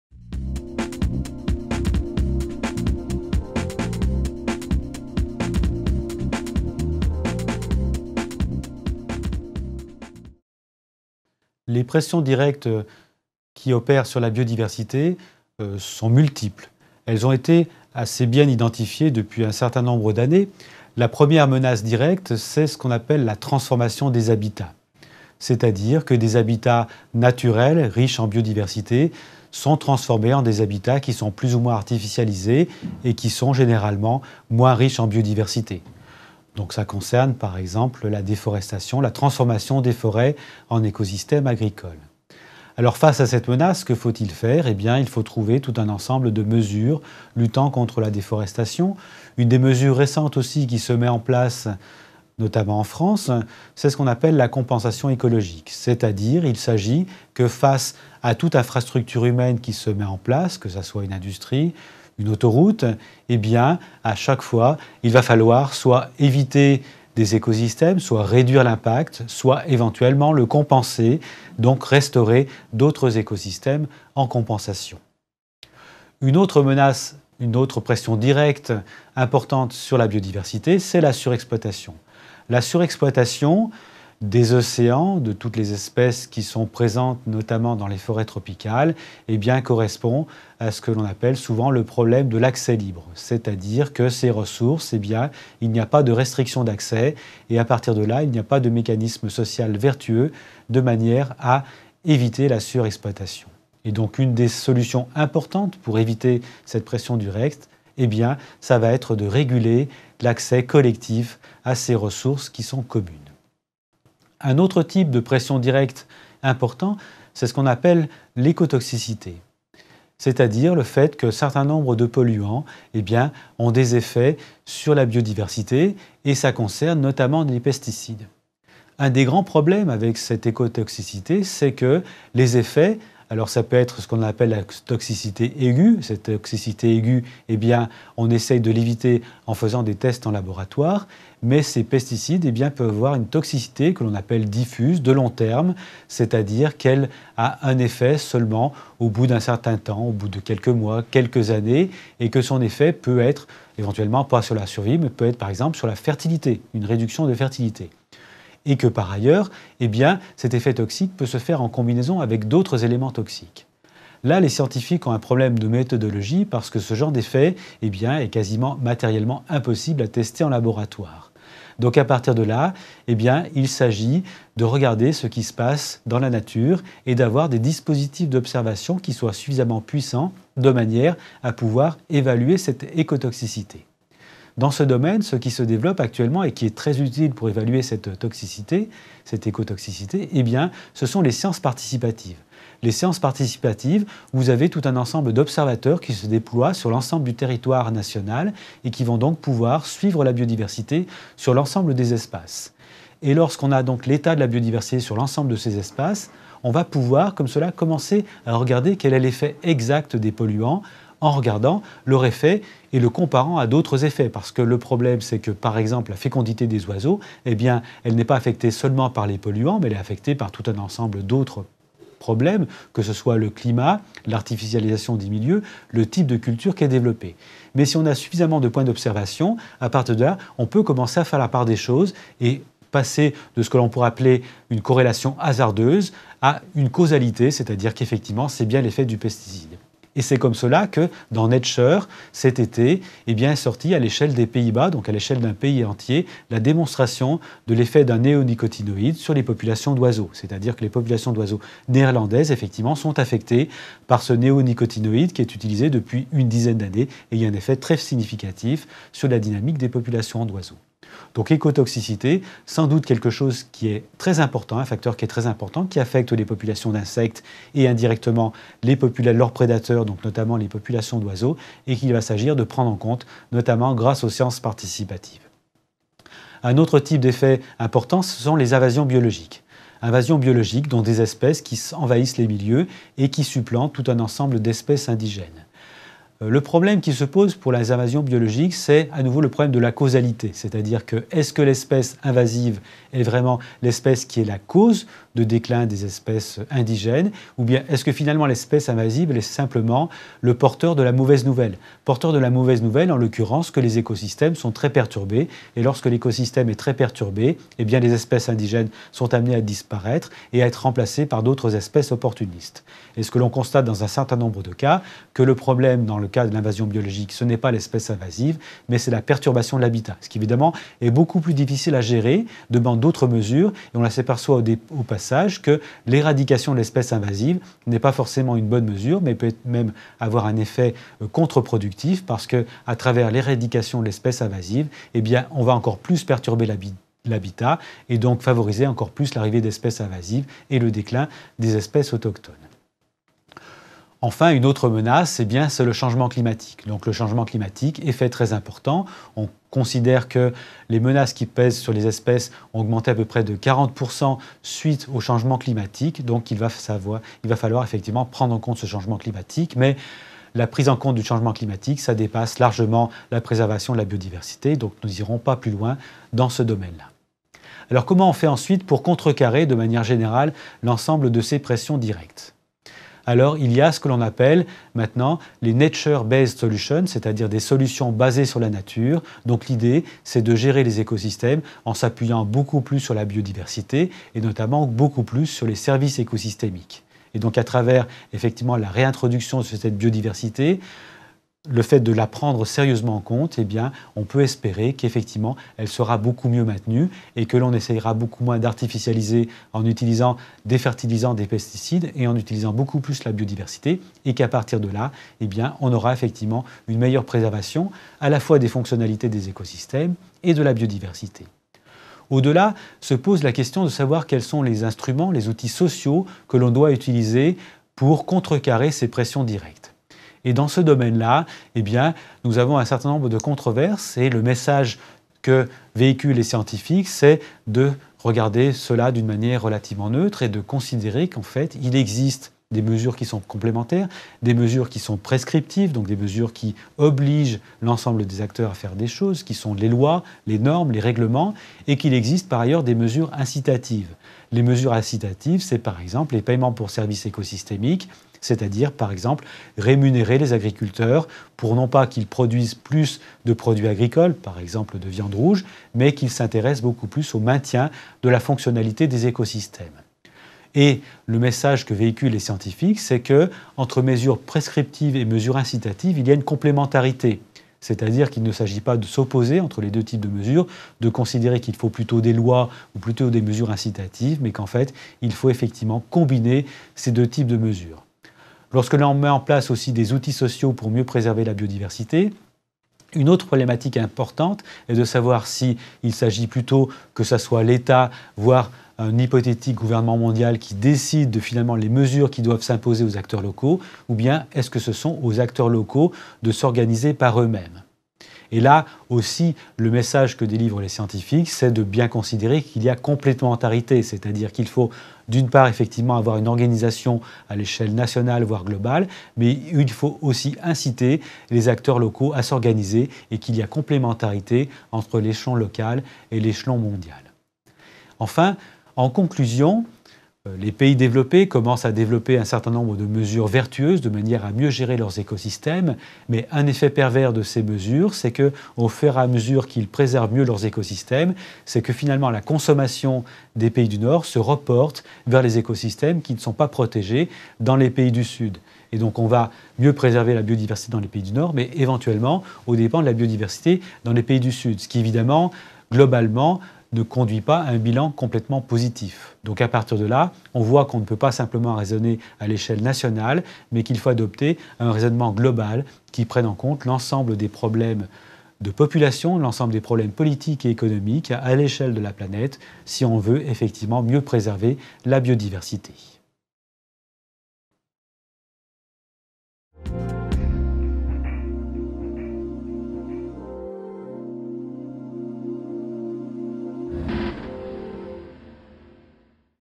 Stratégies de gestion de la biodiversité face aux pressions directes
Denis Couvet propose plusieurs stratégies de gestion de la biodiversité, en réponse aux pressions directes qu'elle subit (destruction, surexploitation, écotoxicité, espèces invasives, changement climatique). Il évoque ainsi la compensation, la régulation, les sciences participatives, ou encore les Nature based Solutions. Afin de déployer cette variété de réponses, il préconise de mobiliser une large palette de politiques, aussi bien prescriptives qu'incitatives.